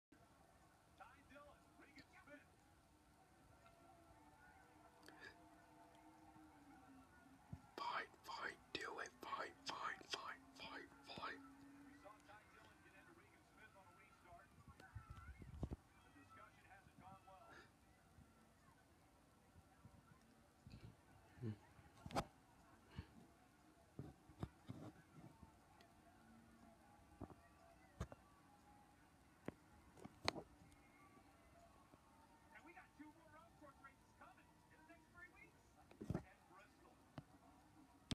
Watkins Glen Fight between Ty Dillon and Regan Smith
This was the fight that took place between Ty Dillon and Regan Smith at Watkins Glen 2 months ago.